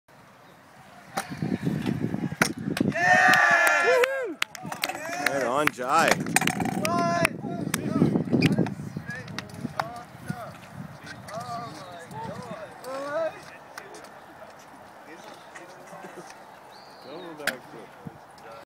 jai walker dubble back flip
Havoc Pro Scooters team rider Jai Walker do this insane dubble back flip in Vernon BC Canada